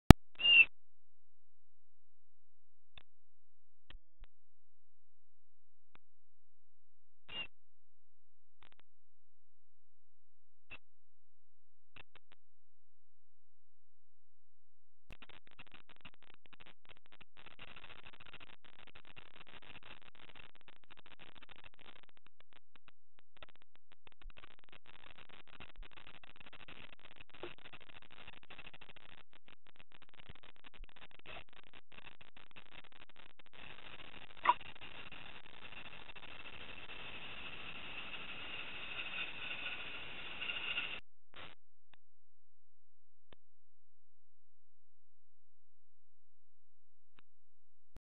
Manatees
Swimming with manatees in Florida.